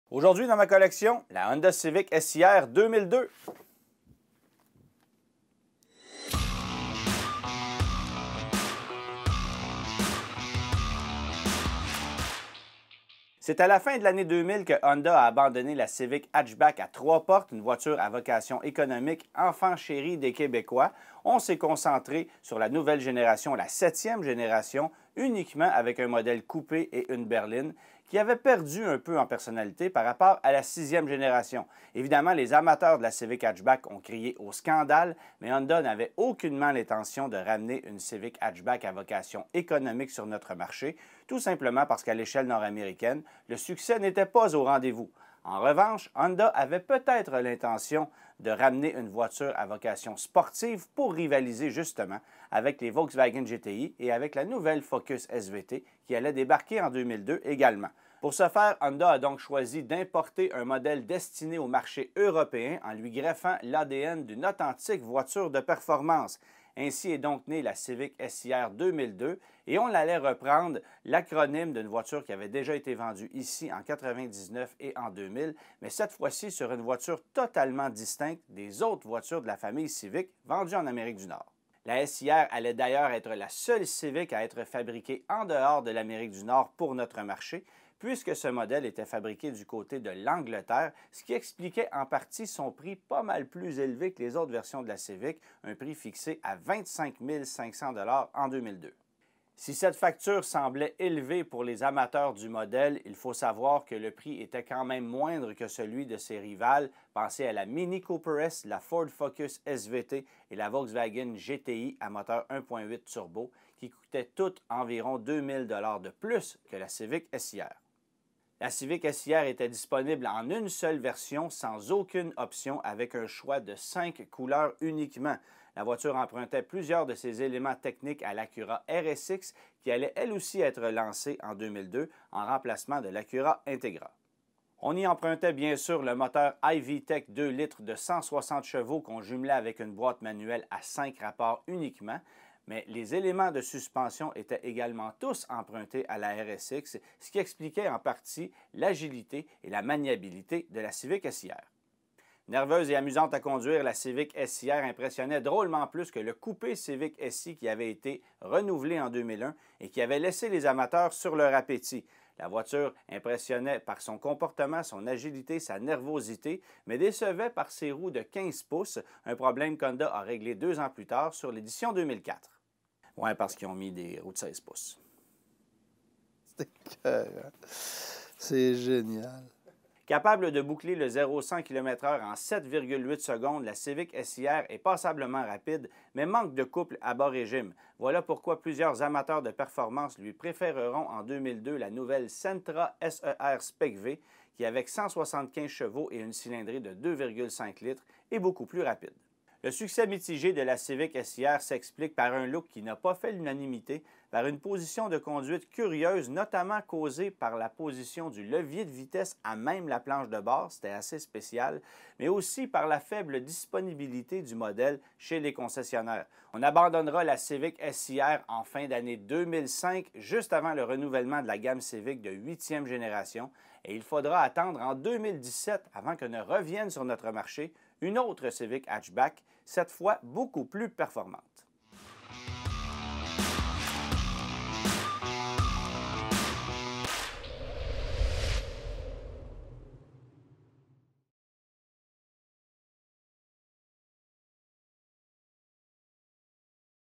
La Honda Civic SiR 2002 - Les 10 001 brochures d'Antoine - E70
Présentation de la Honda Civic SiR 2002 à l'aide de la brochure publicitaire et du document média.